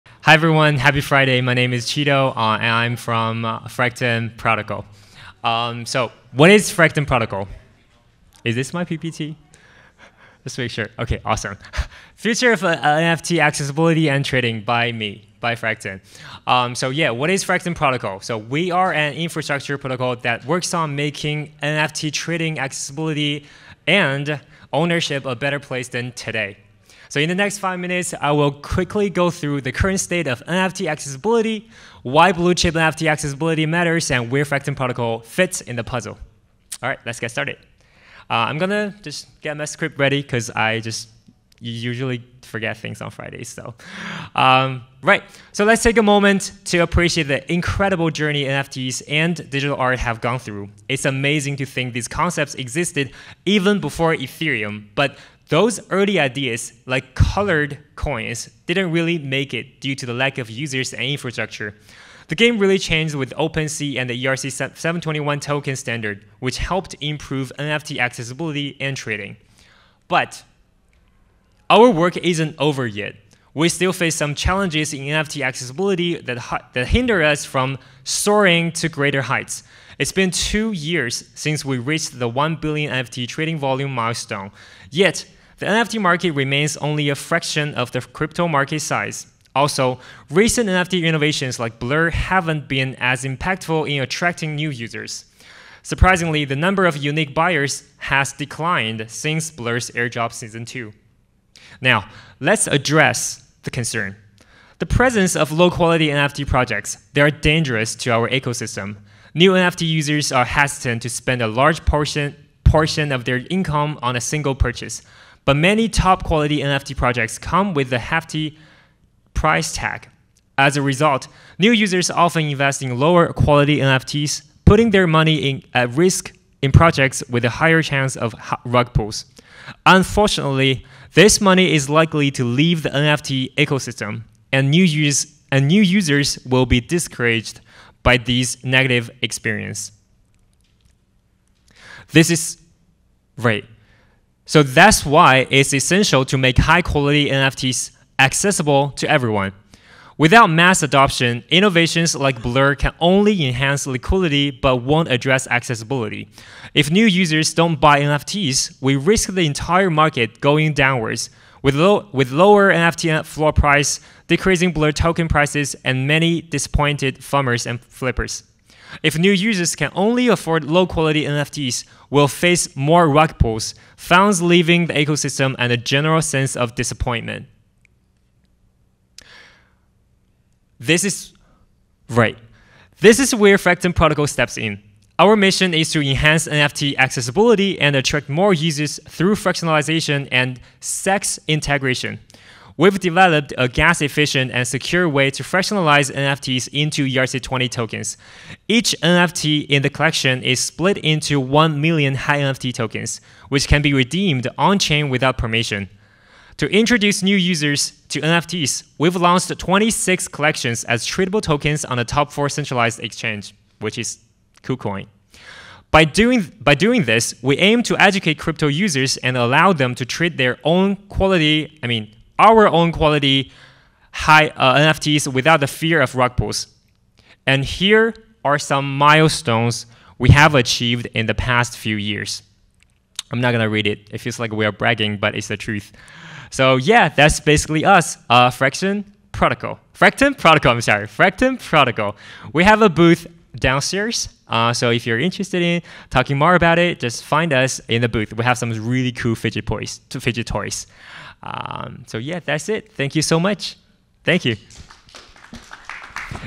The Future of Accessbilty and Liquidity of NFTs - Chido Yan at NFT.NYC 2023
The Future of Accessbilty and Liquidity of NFTs

by Chido Yan

Community Stage

North Javits Center, Community Stage, April 14, 2023